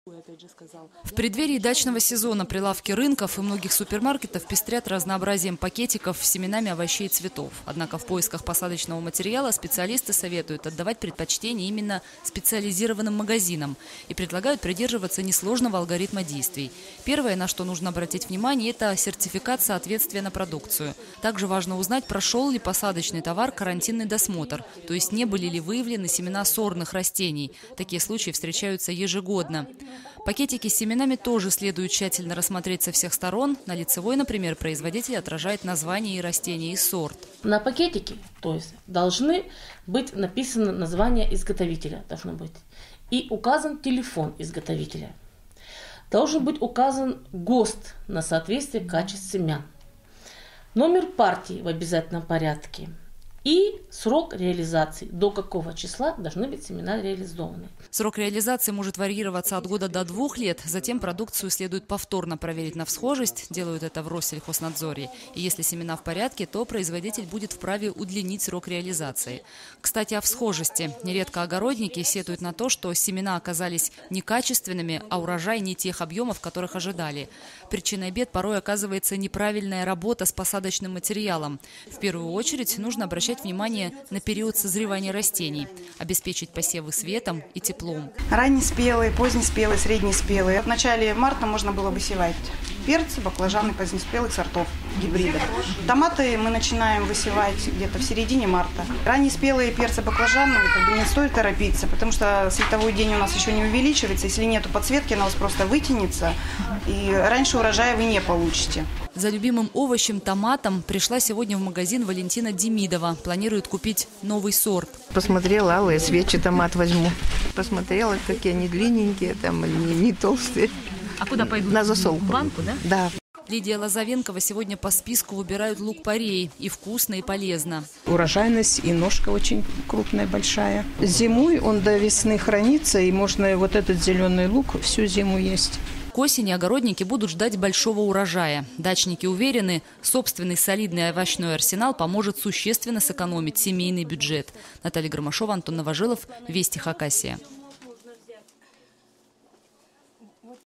Семена пустышки. Как не купить сорняки вместо цветов. 10.03.2016
Снег на улице еще не растаял, однако в магазинах, где продают семена,  уже появились очереди.  Посадочный материал огородники закупают по спискам, и, в первую очередь, берут позднеспелые сорта овощей, ягоды и цветов.  Именно такие можно сеять, начиная с зимы. Как это правильно седлать и, более того,  как не ошибиться в выборе семян?